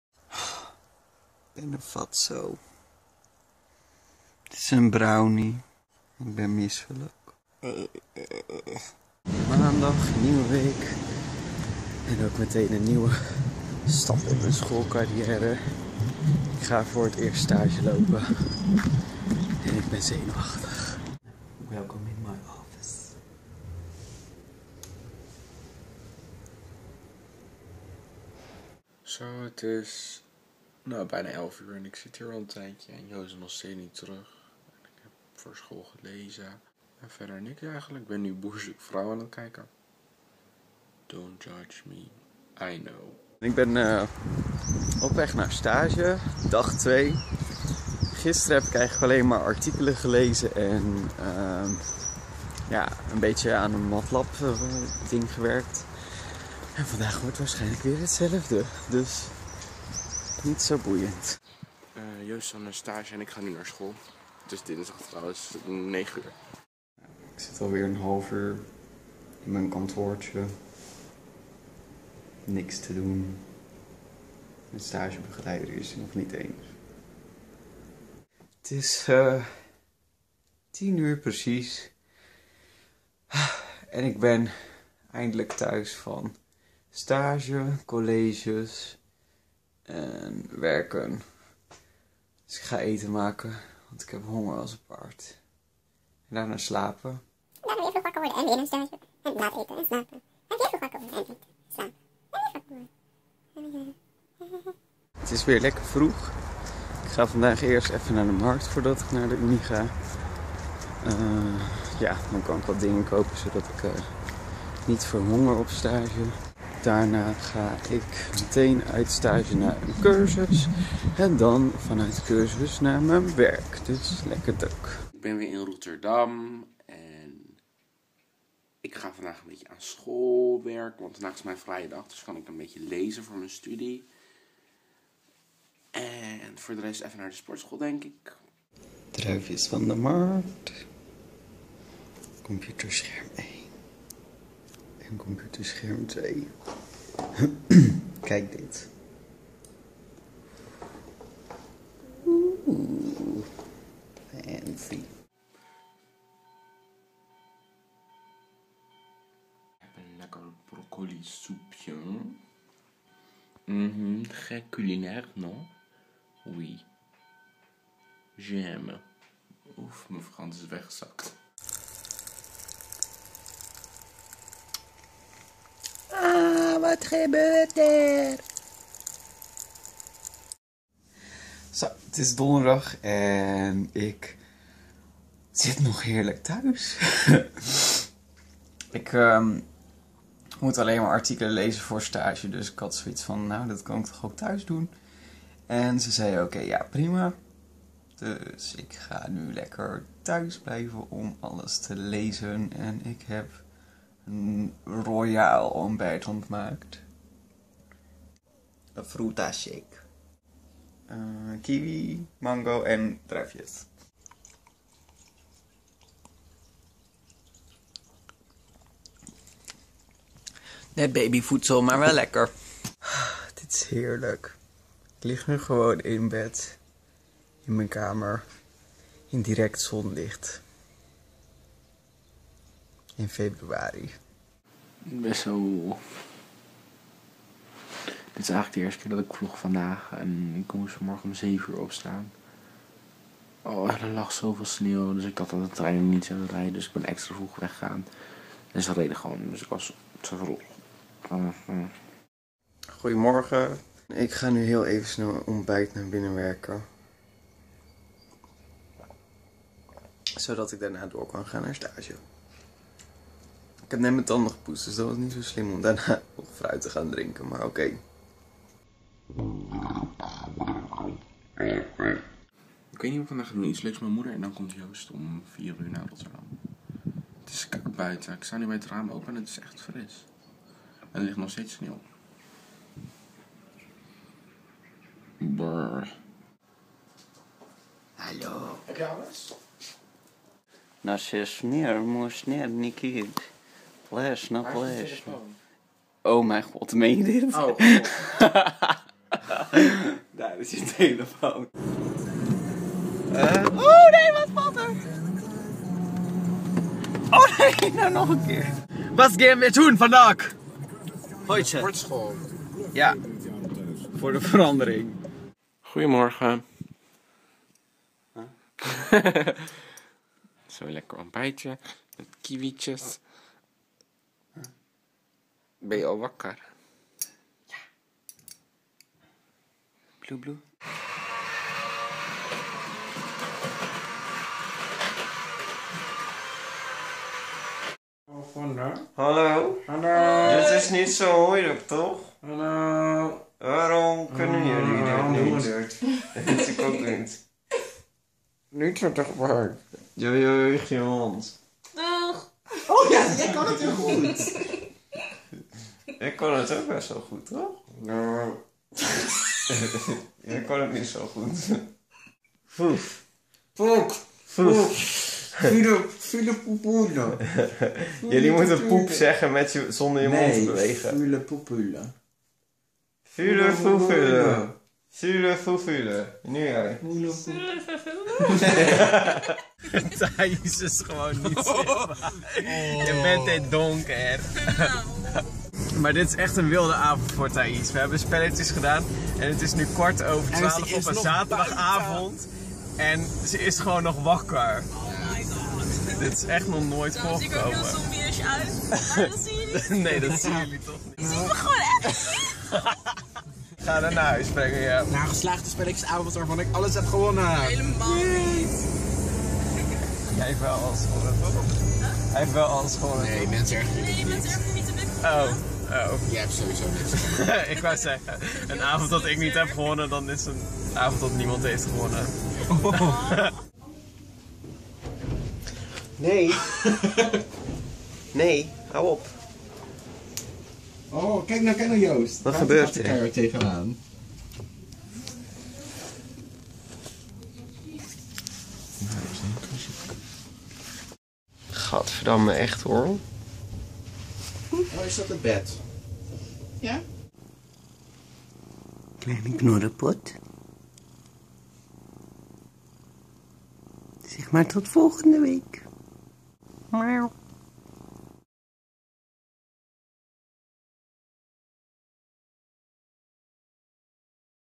week #4 Zeezieke stagedagen
Heee week 4 is hier!!! Een keerpunt in het schoolleven, praten zonder je mond te bewegen en schommelen op een boot... klinkt goed toch?